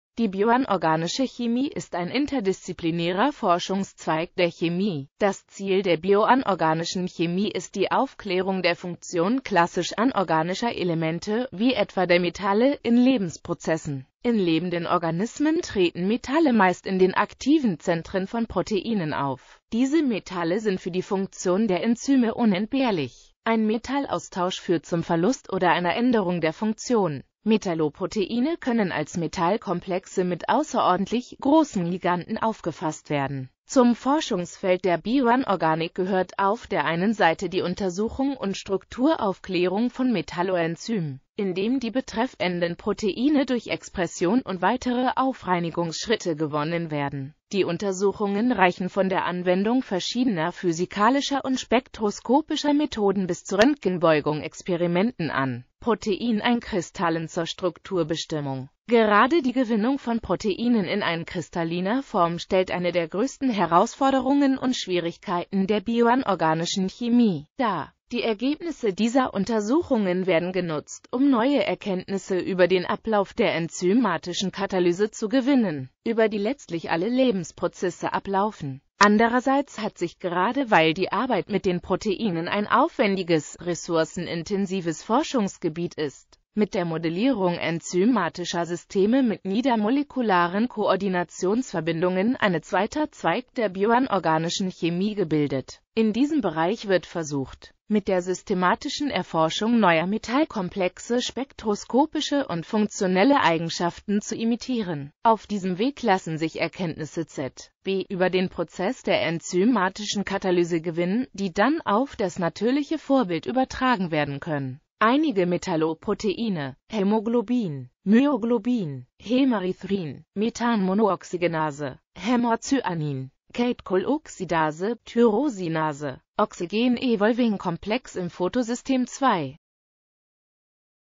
Bioanorganische Chemie
Vielen Dank für Ihre Unterstützung:

Die bioanorganische Chemie ist ein interdisziplinärer Forschungszweig der Chemie.Das Ziel der bioanorganischen Chemie ist die Aufklärung der Funktion klassisch anorganischer Elemente, wie etwa der Metalle, in Lebensprozessen.In lebenden Organismen treten Metalle meist in den aktiven Zentren von Proteinen (Enzymen) auf.Diese Metalle sind für die Funktion der Enzyme unentbehrlich.